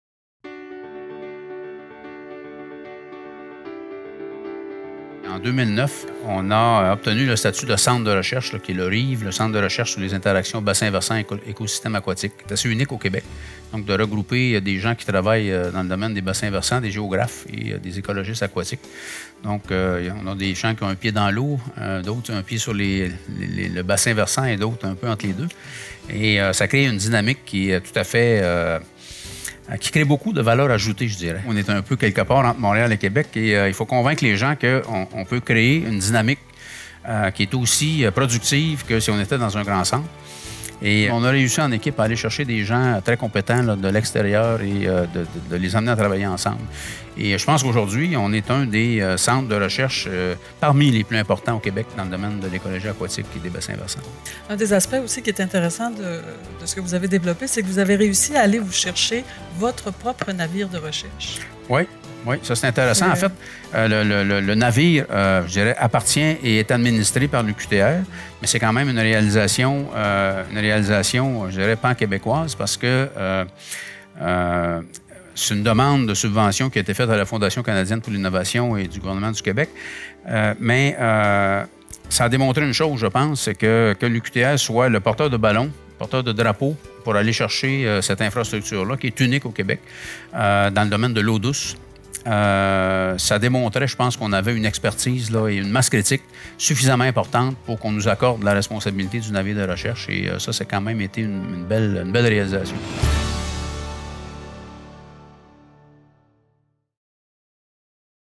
Pierre Magnan remporte le Prix d'excellence en recherche et création, volet Leadership 2016
Professeur au Département des sciences de l’environnement de l’UQTR, Pierre Magnan est titulaire de la Chaire de recherche du Canada en écologie des eaux douces et directeur du Centre de recherche sur les interactions bassins versants — écosystèmes aquatiques (RIVE). Au cours de sa carrière, il a obtenu des organismes subventionnaires plus de 25 M$ pour ses différents travaux de recherche qui lui ont notamment permis de fonder successivement le Laboratoire de recherche sur les communautés aquatiques (LRCA), le Groupe de recherche sur les écosystèmes aquatiques (GRÉA) et le RIVE à l’UQTR. Il a également été un des membres fondateurs du Groupe de recherche interuniversitaire en limnologie et en environnement aquatique (GRIL), un regroupement stratégique du Fonds de recherche du Québec – Nature et technologies. Pour son leadership en recherche et création et sa contribution à la société québécoise, Pierre Magnan se voit attribuer le Prix d’excellence en recherche et création, volet Leadership.